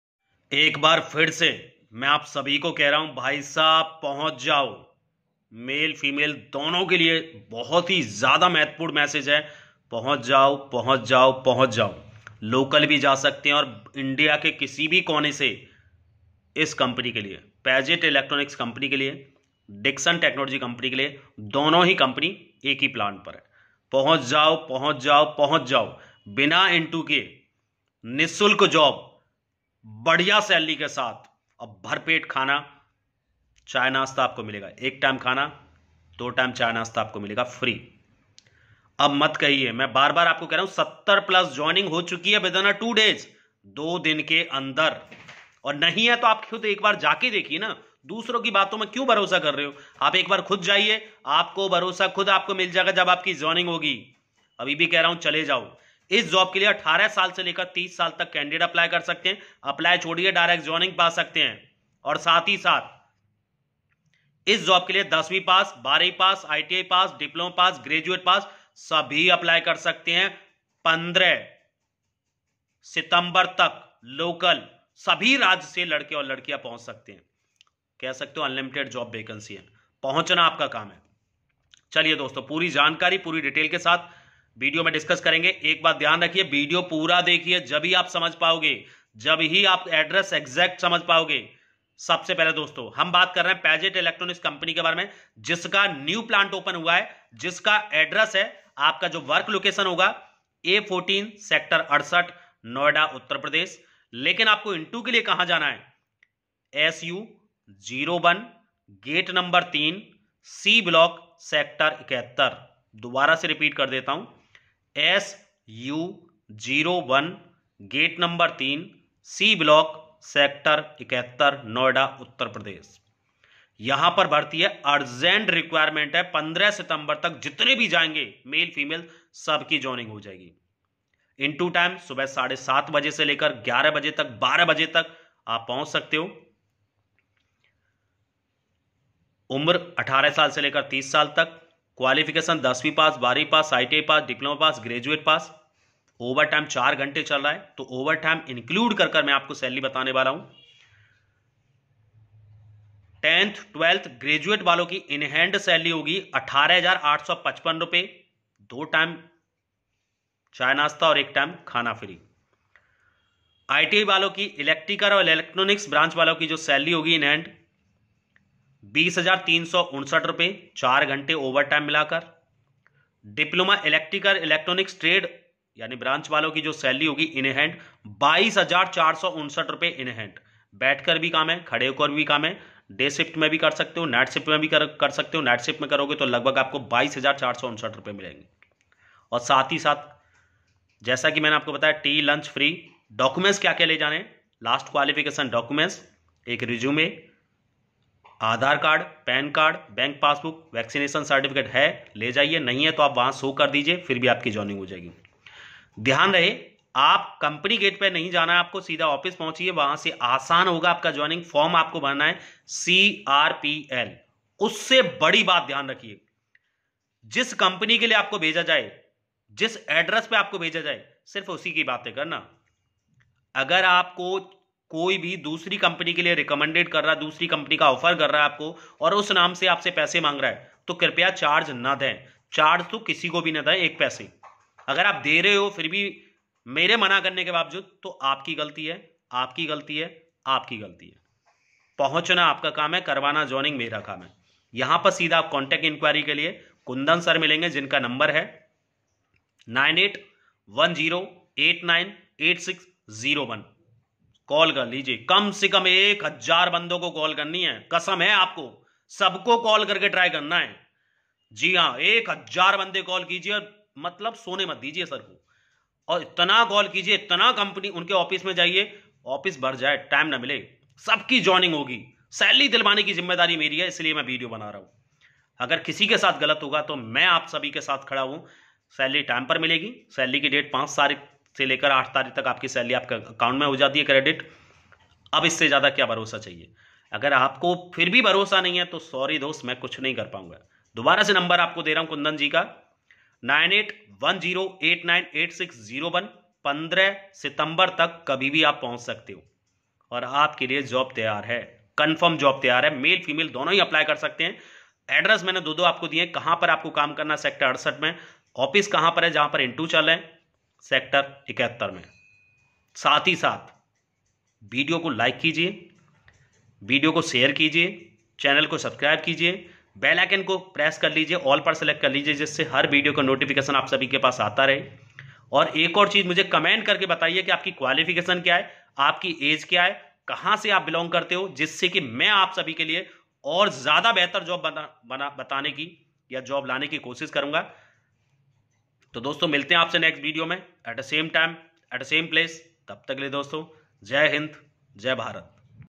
22459 in Hand Salary | Noida Me Badhiya Job Kaise Milegi | How to get Job | Noida Job Update 2023
जॉब पाने के लिए फेसबुक पेज से जुड़े

About This Video - Aaj hum bat karenge Noida me badhiya salary badhiya job kaise paye sath hi over time khana chay nashta sab free milega duty 8 ghante 12 ghante 22459 rupey in hand 12 hours duty male female both can apply direct joining process direct selection process video me full details puri jankari di gyi hai

All Jobs For Over 18yrs